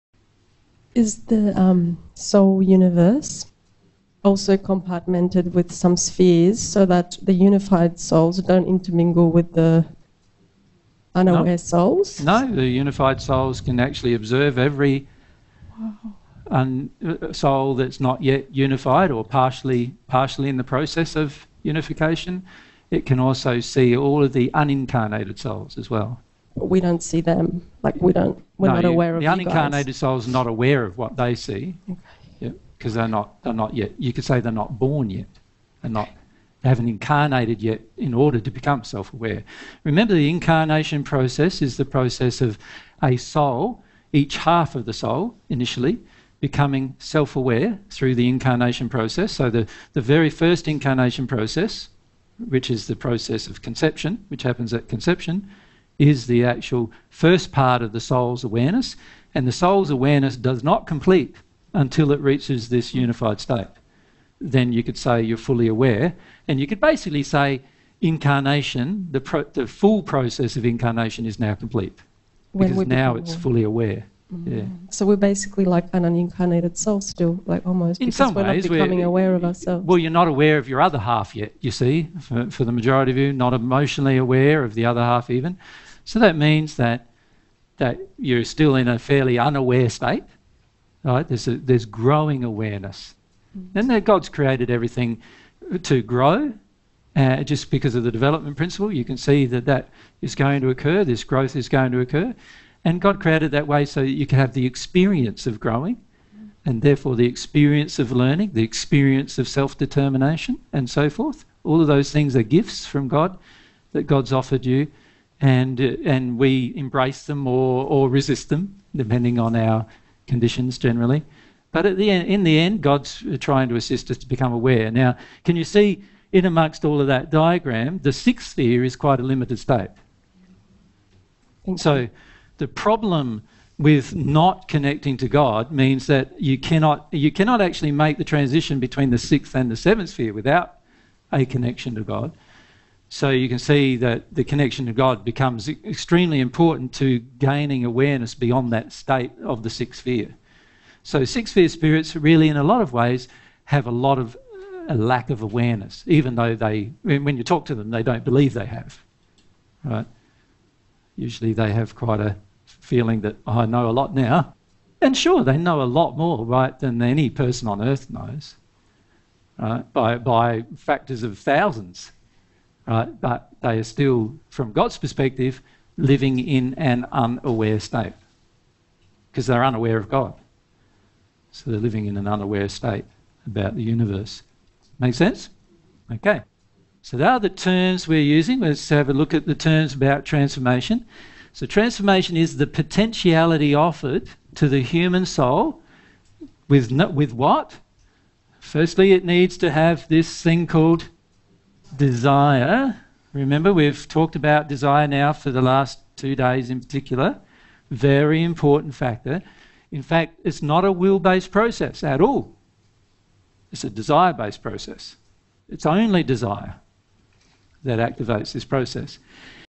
When Is the Soul’s Awareness and Incarnation Fully Complete? Transition Between 6th and 7th Sphere
FULL ORIGINAL:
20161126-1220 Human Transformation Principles

Cut:
10m09s - 13m49s